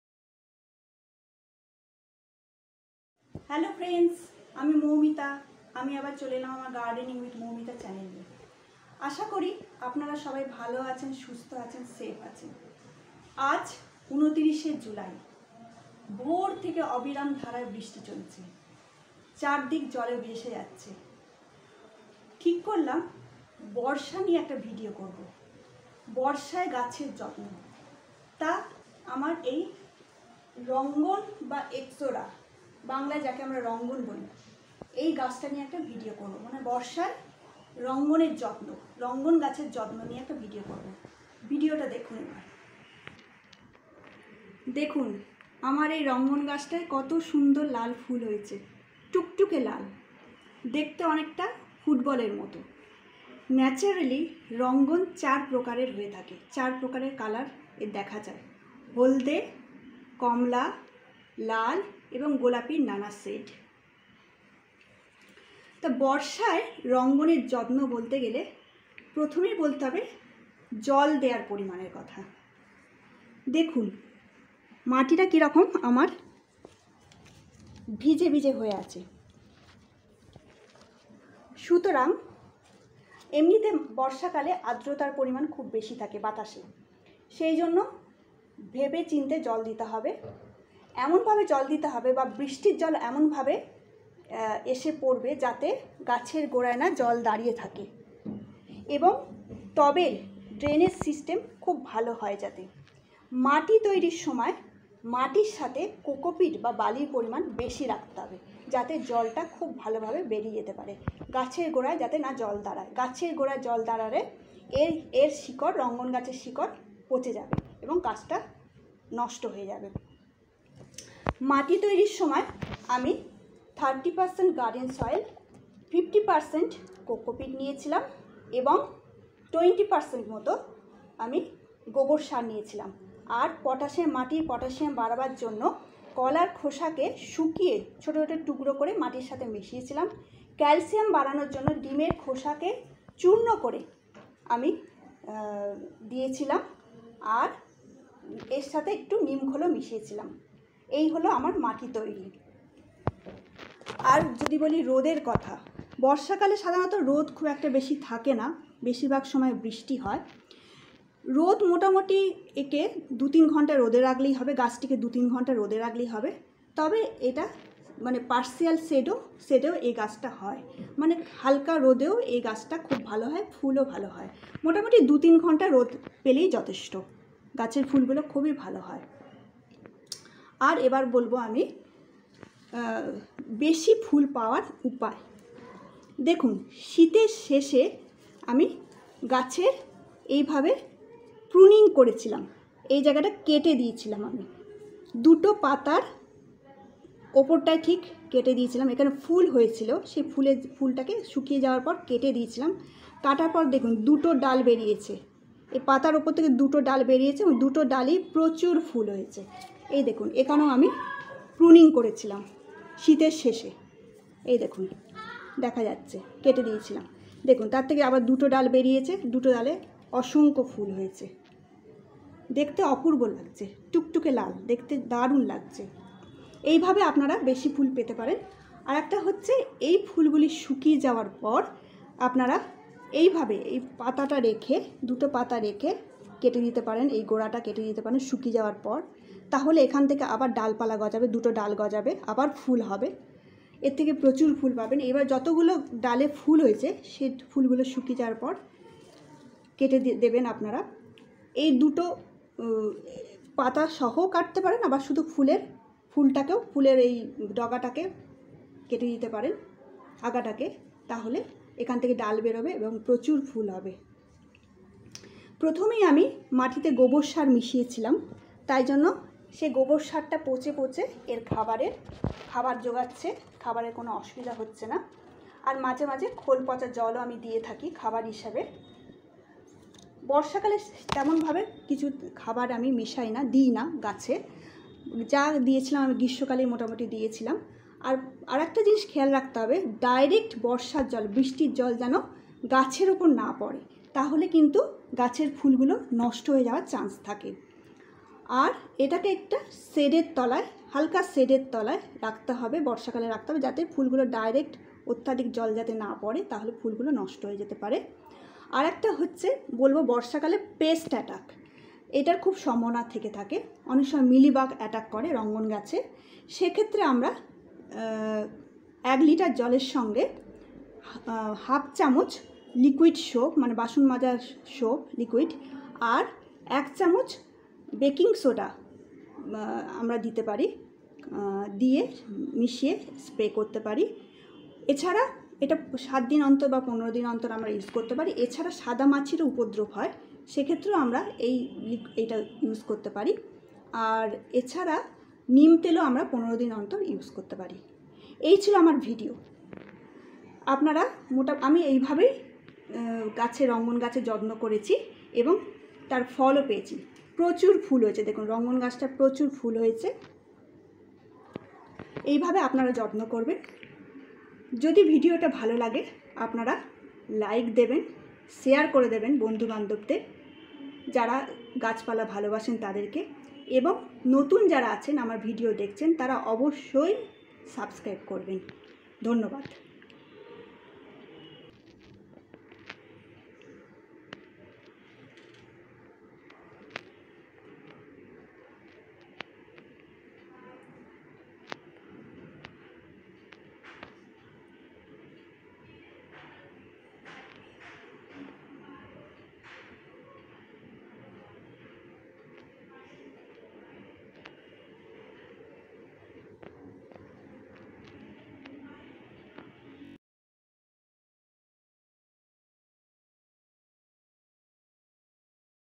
How to get more Flowers,Fertilization and Pruning in Ixora plant in Rainy season
Hello friends,I am back with an amazing video of  how to care Ixora plants.Please watch the video as it can be helpful to you and please be sure to like,share and subscribe.